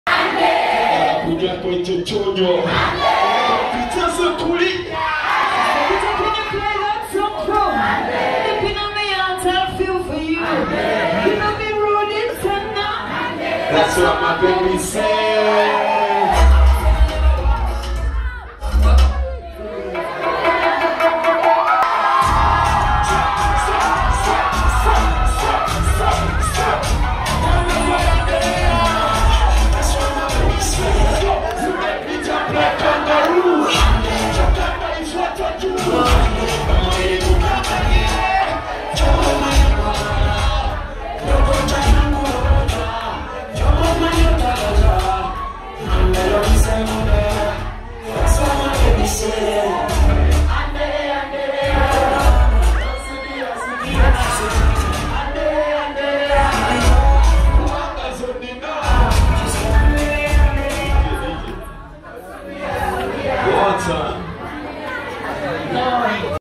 see how @mcenericho almost touched the talent🤪of @NinaRoz mubs DoItForHim performance campgn😜
@mcenericho is one of the best young talent mceez during the DoItForHim/Her campaign he put up a very energetic performance with @NinaRoz Part1